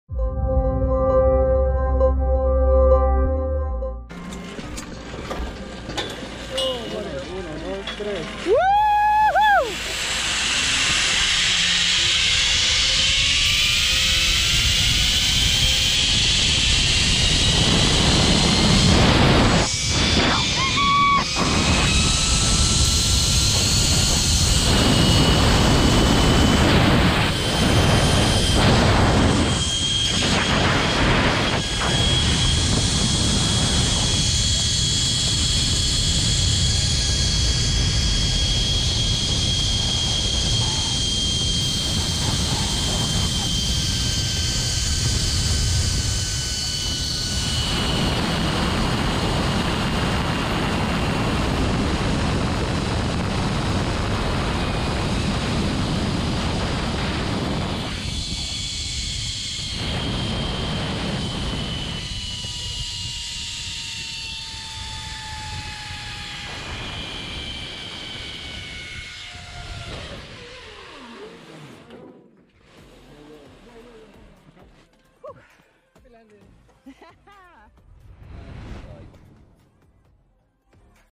Diamante Park superman zipline Costa Rica
Stuart and Eva doing the Superman zipline at the Diamante Eco Park Guanacaste Costa Rica